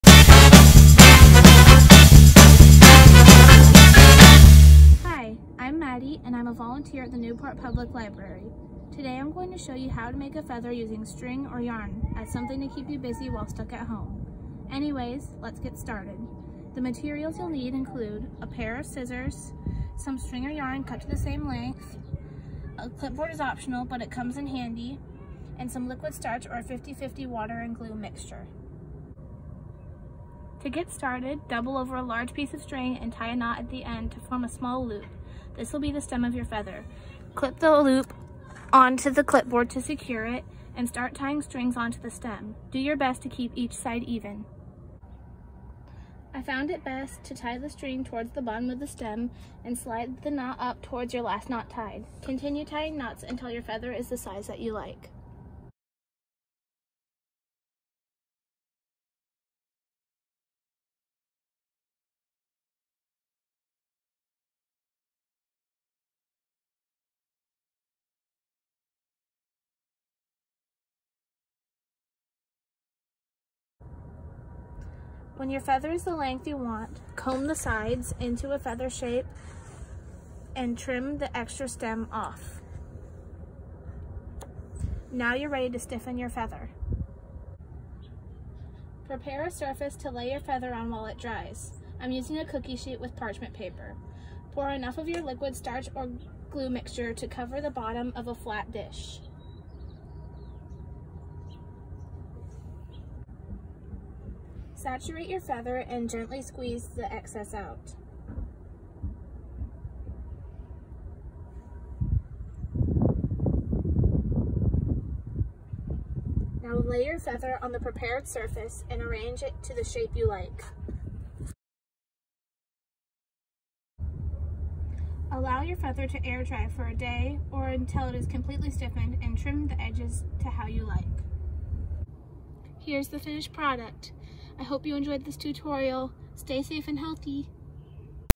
Show and Tell #1--Feather by Maddy--Newport Public Library
Maddy shows us how to make a feather from string and glue!